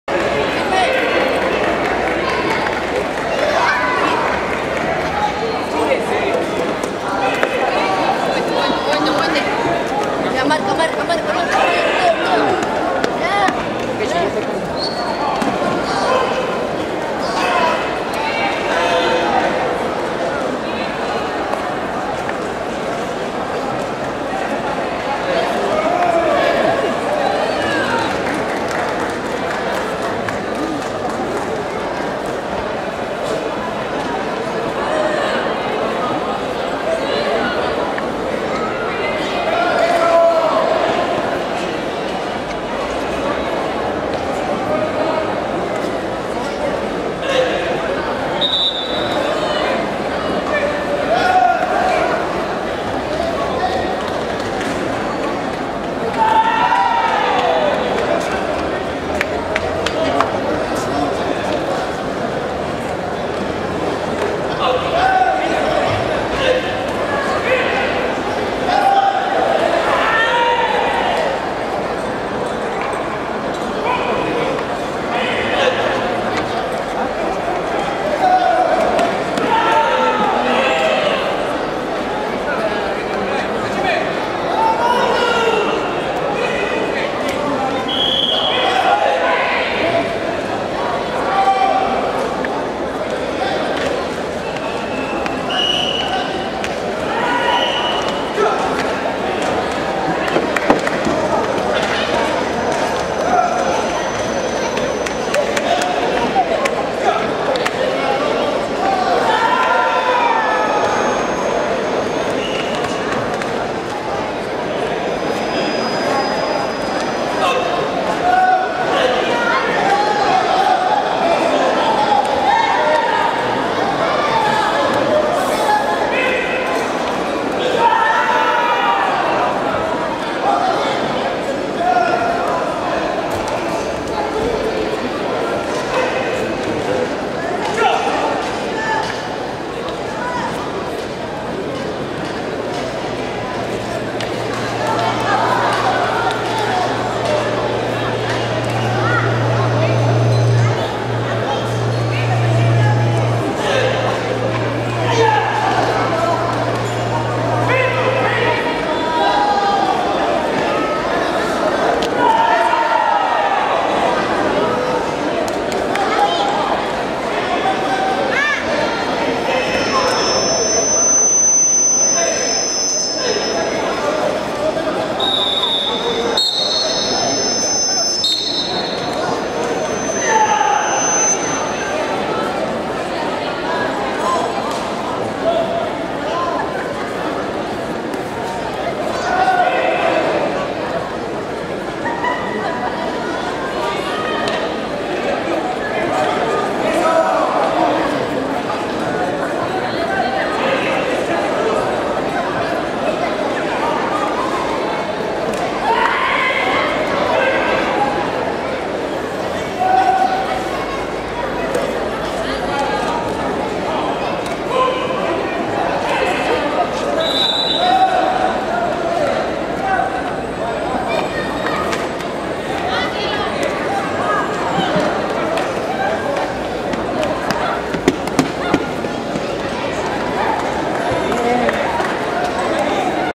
ACADEMIA "NORMA BRITO" CAMPEONATO DE LA FEDERACION PERUANA DE KARATE
Jean Paul Villavicencio en Kumite. Domingo 08 de Diciembre del 2013. Coliseo Dibós.